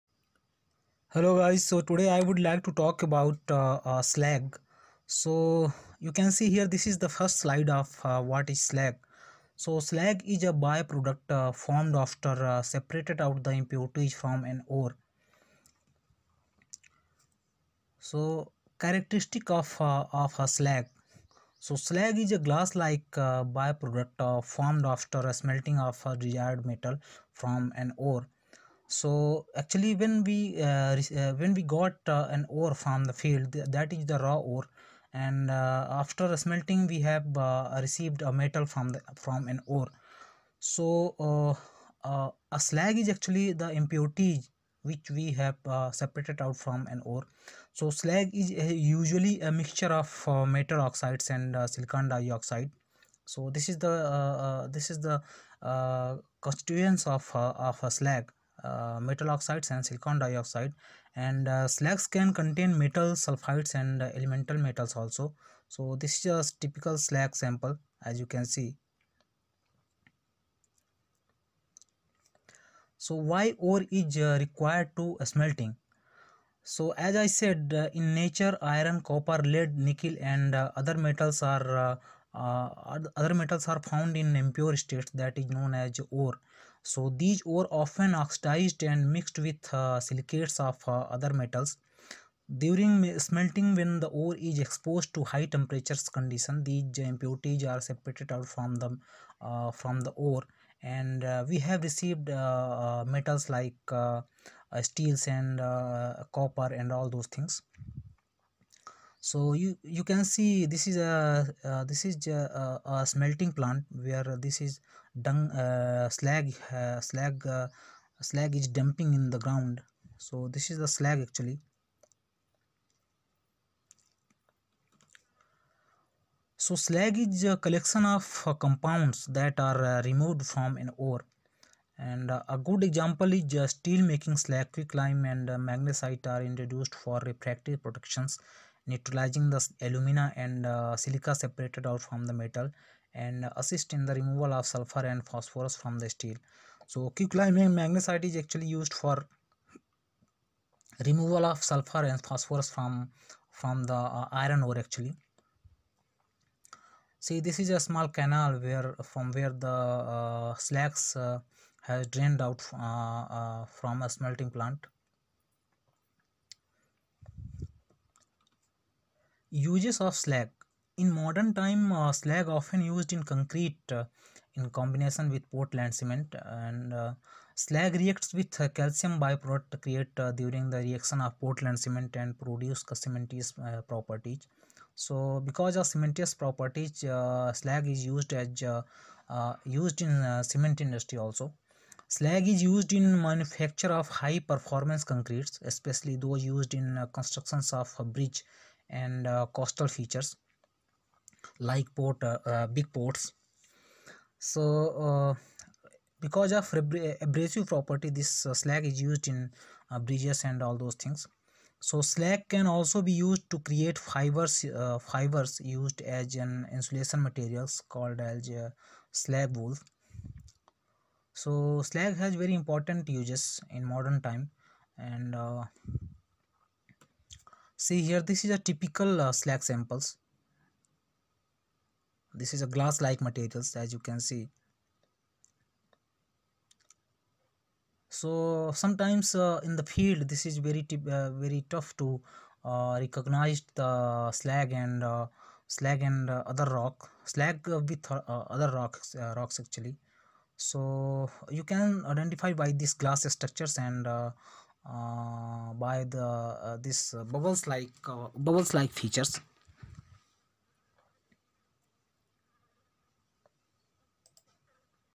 Slag/What is Slag/uses of Slag
Slag is the glass-like by-product left after a desired metal has been separated or smelted from its raw ore. Slag is usually a mixture of metal oxides and silicon dioxide.  Slags can contain metal sulfides and elemental metals. In nature, iron, copper, lead, nickel and other metals are found in impure states called ore, often oxidized and mixed in with silicates of other metals.  During smelting, when the ore is exposed to high temperatures, these impurities are separated from the molten metal and can be removed. So, Slag is the collection of compounds that are removed.

A good example is steel making slag-quicklime (CaO) and magnesite (MgCO3) are introduced for refractory protection, neutralizing the alumina and silica separated from the metal, and assist in the removal of sulfur and phosphorus from the steel
Uses of Slag

In modern time Slag often used in concrete in combination with Portland cement as part of a blended cement. Ground granulated slag reacts with a calcium byproduct created during the reaction of Portland cement to produce cementitious properties. Slag is used in the manufacture of high-performance concretes, especially those used in the construction of bridges and coastal features.Its low permeability and greater resistance to chlorides and sulfates can help to reduce corrosive action and deterioration of the structure. The slag can also be used to create fibers used as an insulation material called slag wool.